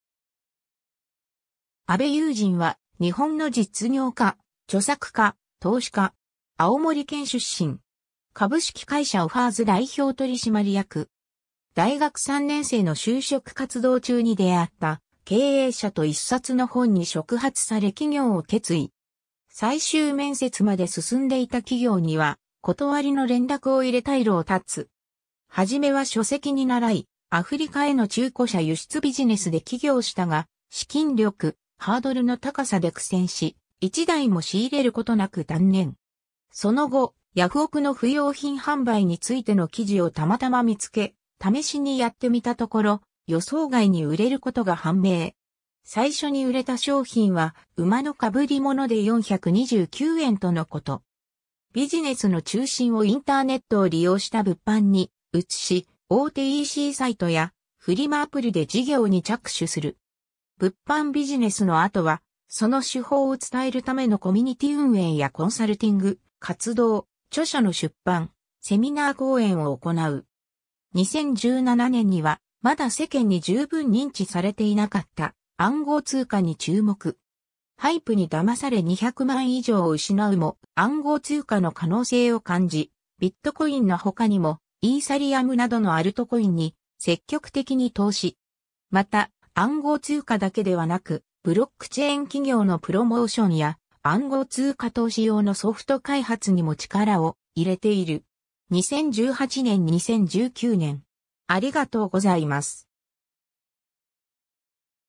阿部悠人
阿部 悠人（あべ ゆうと、1992年 - ）は、日本の実業家、著作家、投資家。
青森県出身。
株式会社オファーズ代表取締役。
 大学3年生の就職活動中に出会った経営者と1冊の本(プータロー、アフリカで300億円、稼ぐ)に触発され起業を決意。
最終面接まで進んでいた企業には断りの連絡を入れ退路を断つ。
初めは書籍に習い、アフリカへの中古車輸出ビジネスで起業したが、資金力、ハードルの高さで苦戦し、1台も仕入れること無く断念。
その後、ヤフオクの不用品販売についての記事をたまたま見つけ、試しにやってみたところ、予想外に売れることが判明。
最初に売れた商品は「馬の被り物」で429円とのこと。
ビジネスの中心をインターネットを利用した物販に移し、大手ECサイトやフリマアプリで事業に着手する。
物販ビジネスの後は、その手法を伝えるためのコミュニティ運営やコンサルティング活動、著書の出版、セミナー講演を行う。
 2017年には、まだ世間に十分認知されていなかった暗号通貨に注目。
HYPEに騙され200万以上を失うも、暗号通貨の可能性を感じ、ビットコインの他にもイーサリアムなどのアルトコインに積極的に投資。
また、暗号通貨だけではなく、ブロックチェーン企業のプロモーションや暗号通貨投資用のソフト開発にも力を入れている。
 2018年 2019年